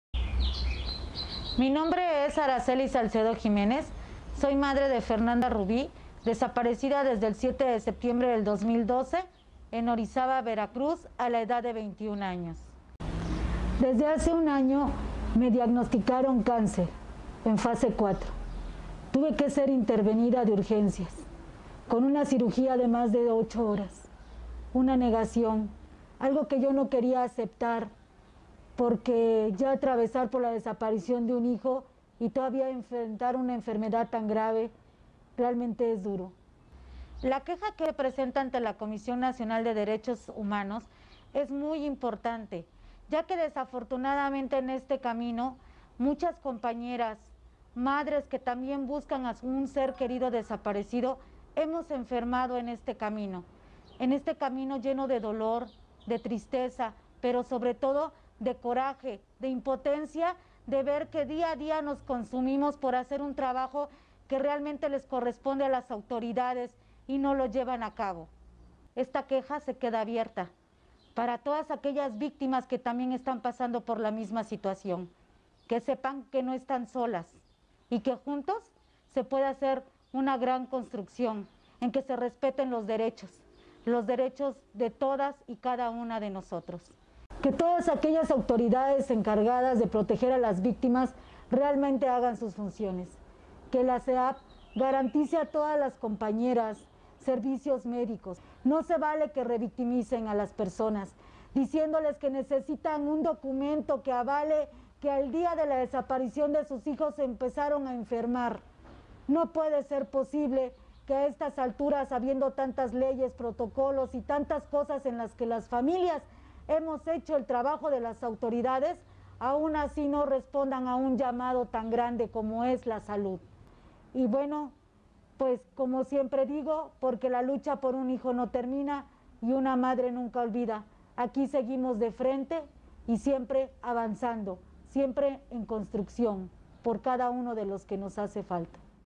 Familiares presentan queja contra CEAV por violación de su derecho a la salud
Testimonio de Araceli Salcedo, madre de Fernanda Rubí, desaparecida en Orizaba Veracruz desde el 07 de septiembre de 2012.

Las víctimas –siete mujeres y cuatro hombres de entre 48 y 84 años de edad– han desarrollado graves enfermedades y padecimientos de salud luego de la desaparición de sus familiares, como cáncer, diabetes y glaucoma. En un caso, el padre de una de las personas desaparecidas falleció de cáncer en 2019 sin ninguna atención médica por parte de la CEAV. La queja destaca que dos de ellas desarrollaron problemas de salud derivados de sus actividades de búsqueda.